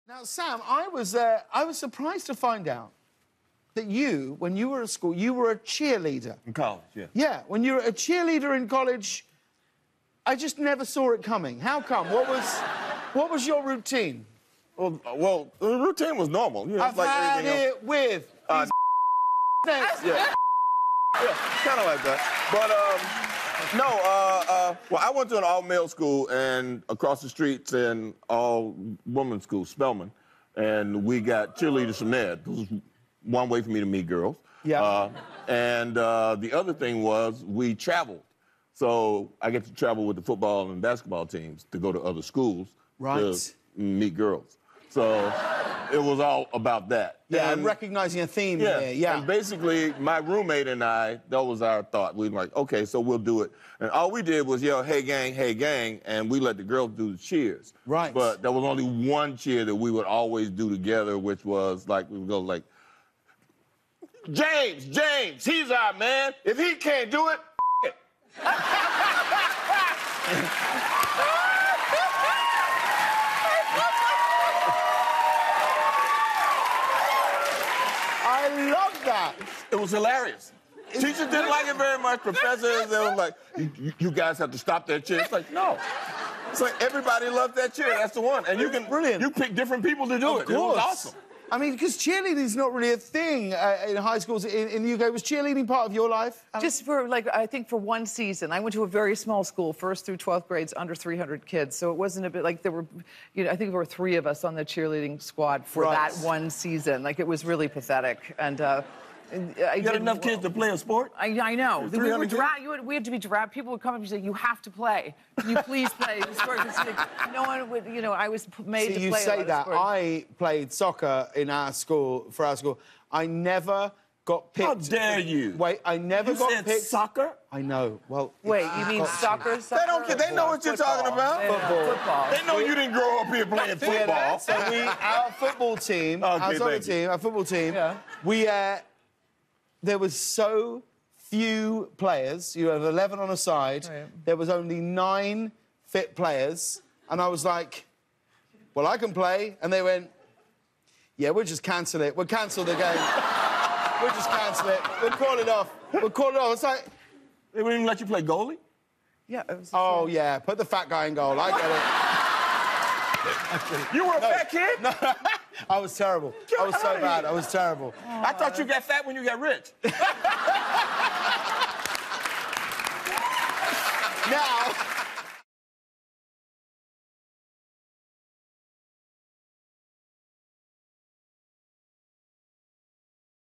Former Cheerleader Samuel L. Jackson Has a Cheer for James
James asks Samuel L. Jackson about his days as a college cheerleader, for which he has a simple explanation. And Samuel shares his favorite cheer that pretty much works for any name in the book.